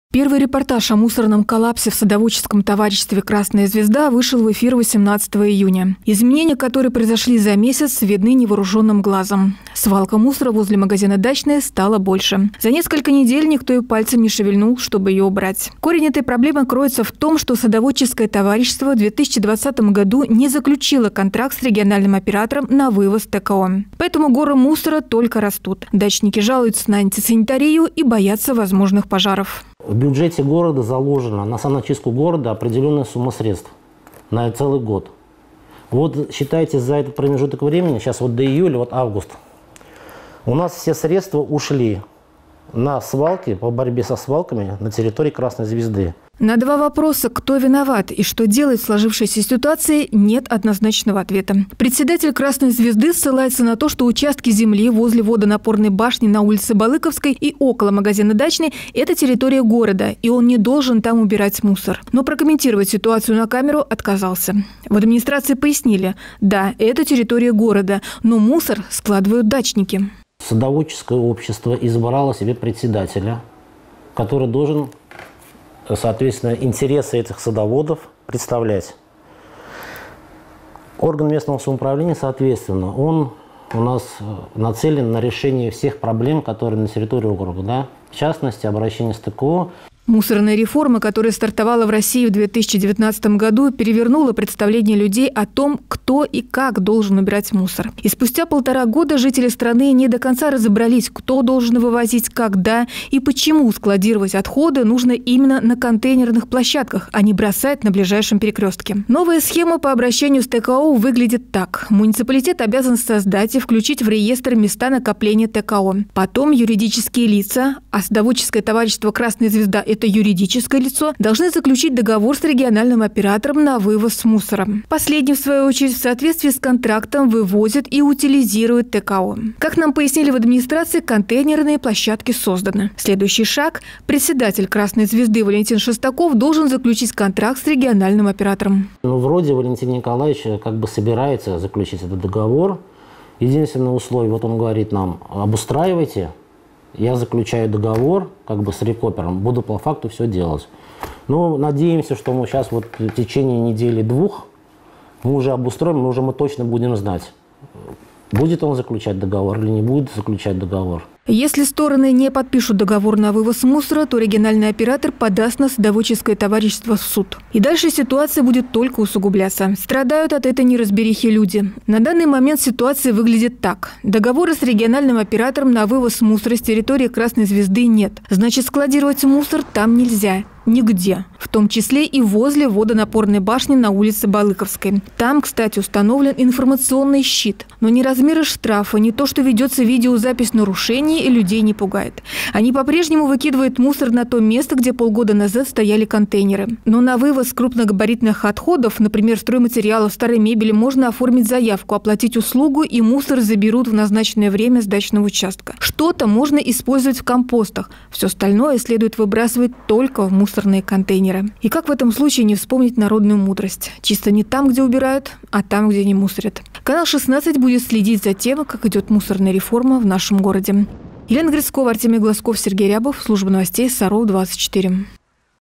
Деньги на мусор?
Первый репортаж о мусорном коллапсе в садоводческом товариществе «Красная звезда» вышел в эфир 18 июня. Изменения, которые произошли за месяц, видны невооруженным глазом - свалка мусора возле магазина «Дачный» стала больше. В ситуации продолжают разбираться журналисты «Канала-16».